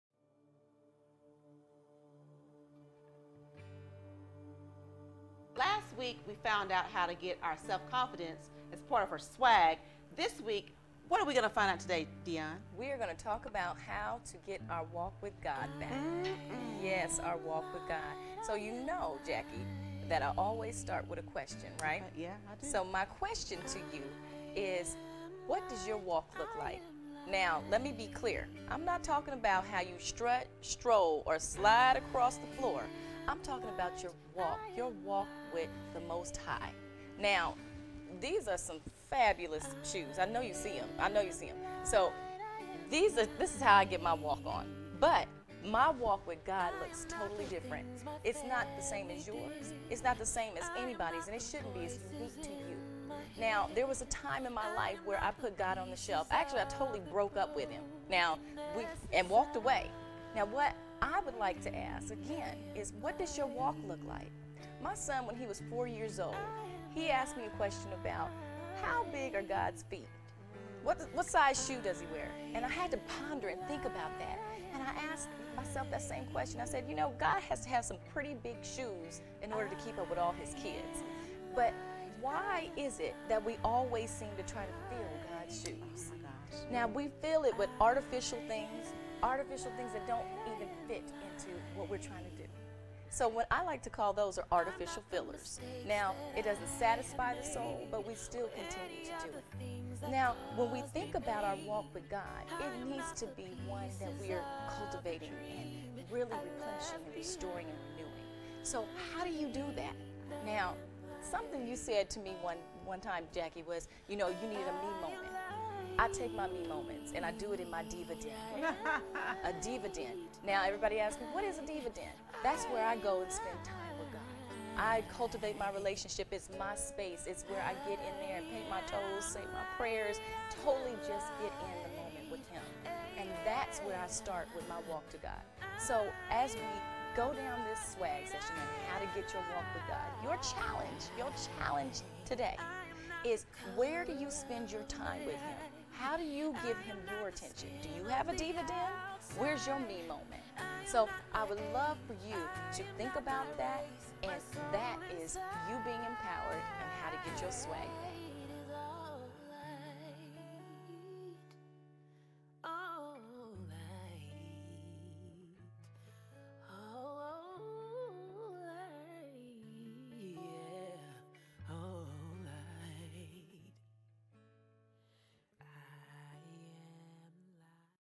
Girl, Get Your S.W.A.G. Back! Restoring Your Walk with God
This video is a brief highlight of the second stop on the soul-freeing S.W.A.G. journey and is designed to help you examine your spiritual walk with our Heavenly Father. This part of the journey is the very crux for getting your S.W.A.G. back!  You can't spell S.W.A.G. without the "W" or you will have S.A.G. Our S.W.A.G. can not SAG! That is not cute! This means we can't live our life on purpose or to the fullest with out a stable and consistent walk with God.  For more information about "Girl, Get Your S.W.A.G. Back!", please

DeYonne Parker is a published author, professional speaker and coach. Connect with her!